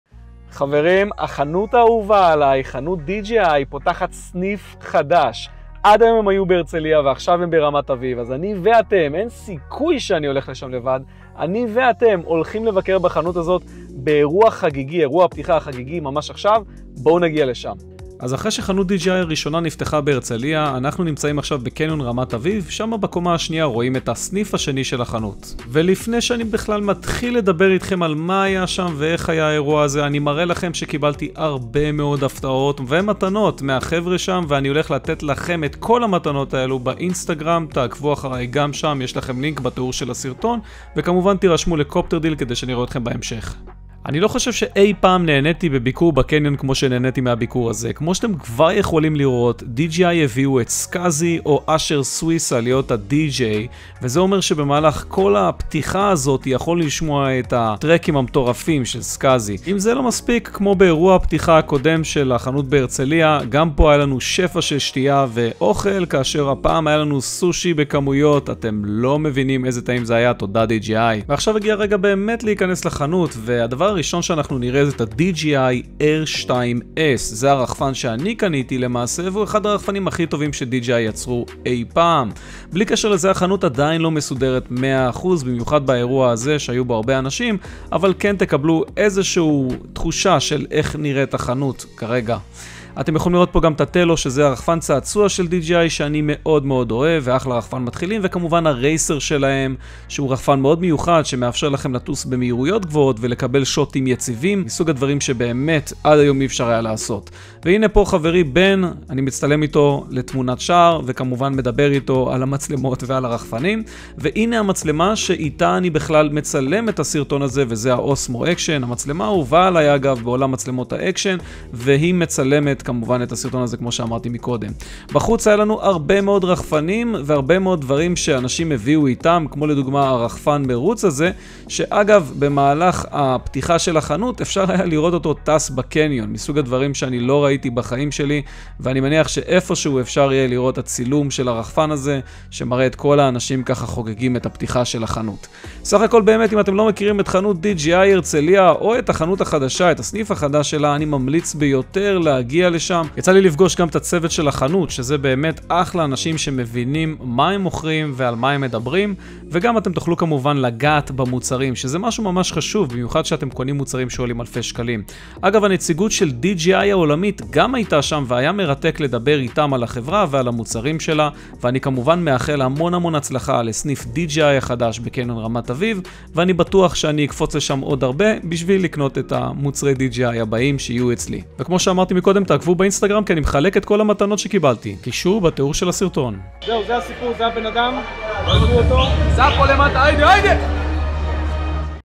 חנות רחפנים חדשה בישראל?!
אנחנו יוצאים שוב מהאולפן כי לא כל יום החנות האהובה עליי בישראל פותחת סניף חדש - אנחנו כמובן מדברים על חנותDJI והסניף החדש ממוקם בקניון רמת אביב בתל אביב ואנחנו נבקר בו ביחד היום בסרטון